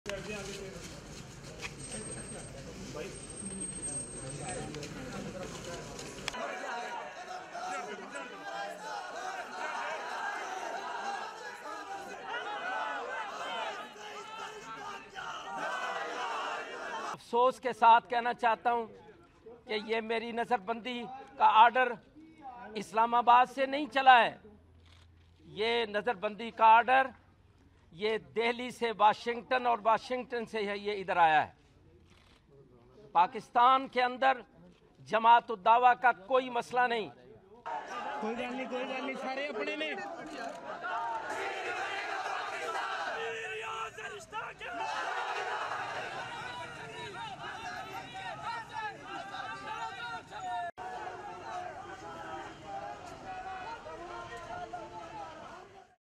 Pakistan cracks down on group linked to Mumbai attacks
Pakistan's police place a firebrand cleric linked to the 2008 Mumbai attacks under house arrest after the government issue a directive following years of pressure to act. Hafiz Saeed, who heads the Jamaat-ud-Dawa (JuD) group and has a $10 million US bounty on his head, is to be placed under "preventative detention", according the order from the interior ministry.